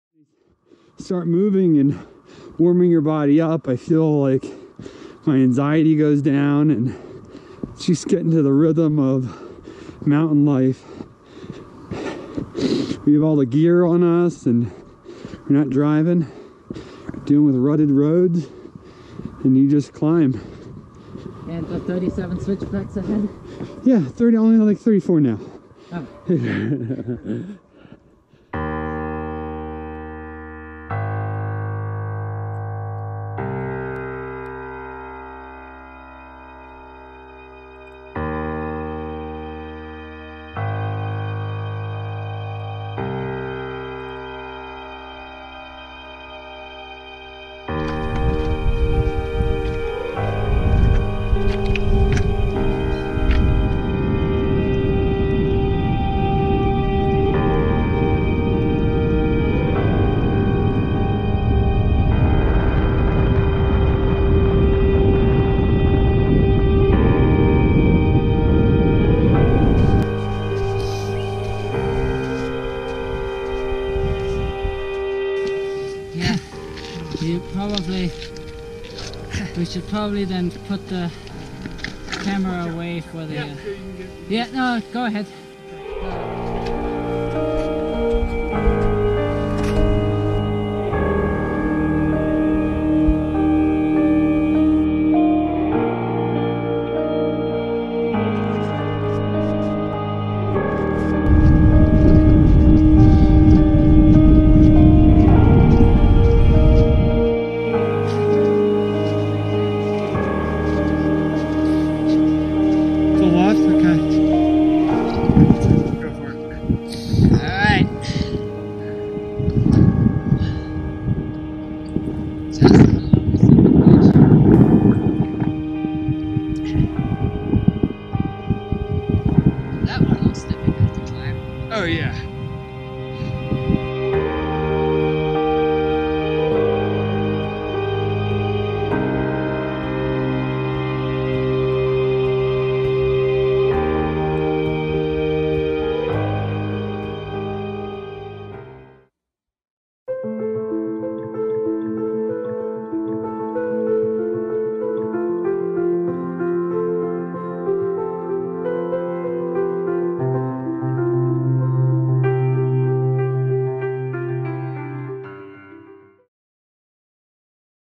Sahale Peak 2.0
Three years ago I attempted to climb Sahale Peak, but failed due to a lack of gear while attempted to scramble up the glacier. This past weekend I returned with a friend to ascend once again. While a snow-covered summit stack meant I fell 30' short of the summit, I scored the day up as a win nevertheless.